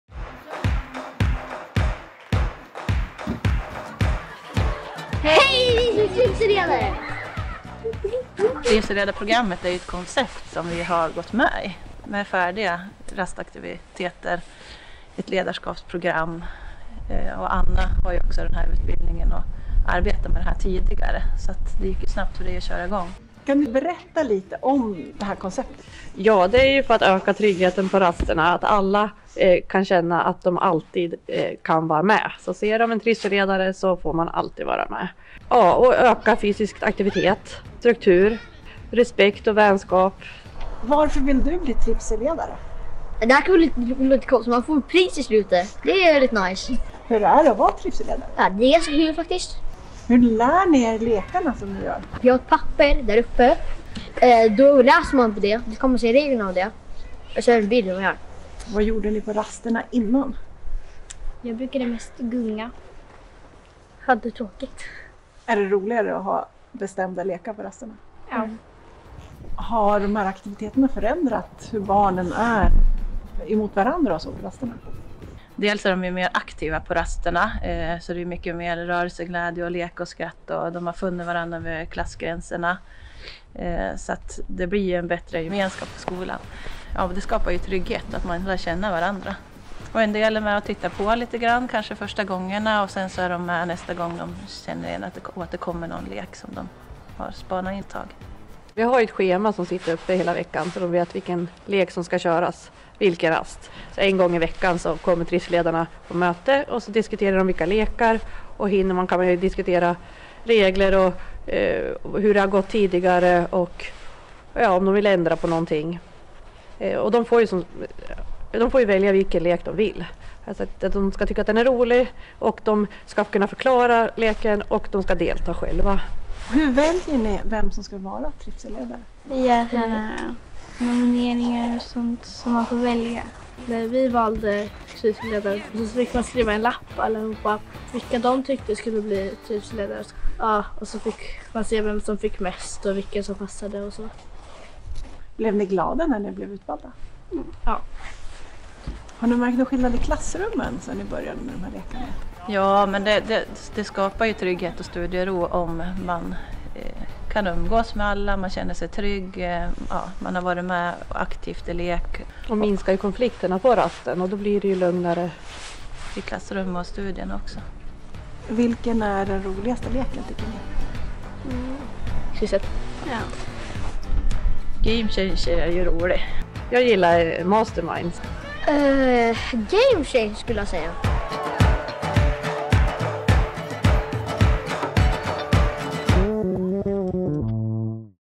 Trivsellekar på rasten
Film för Nordanstigs kommun om rastaktiviteter på Gnarps skola